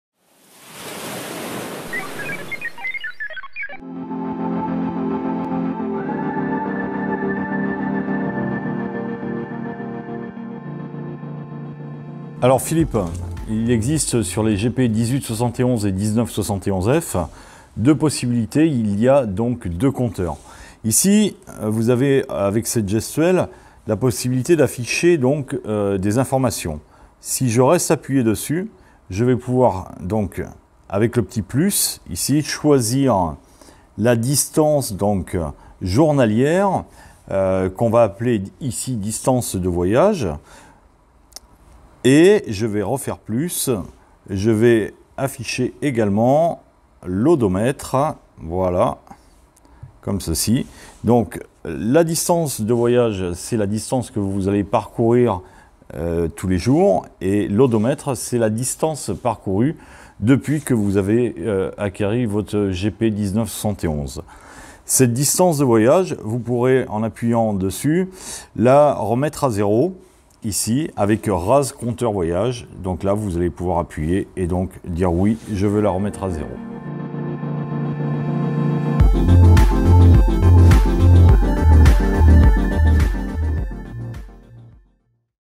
Comment savoir le nombre total de milles parcourus dans la journée ? Combiné Sondeur GPS | GP-1X71F
Furuno France répond aux questions de ses utilisateurs : Comment savoir le nombre total de milles parcourus dans la journée ?

Plus d'infos sur nos combinés sondeur GPS GP-1871F et GP-1971F